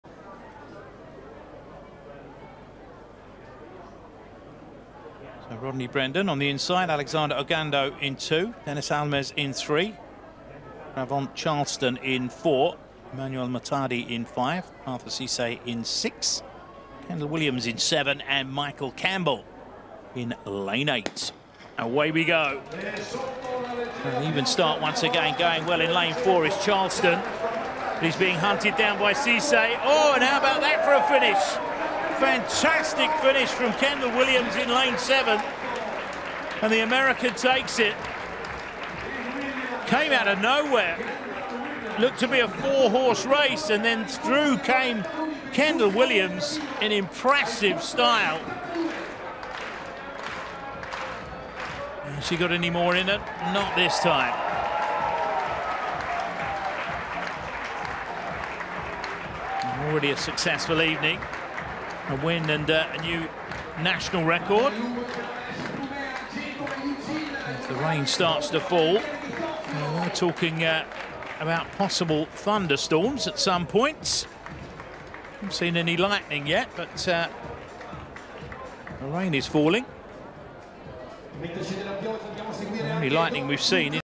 Kendal Williams Takes Men's 100m With Late Burst
Williams ran 10.08 to win in Rovereto.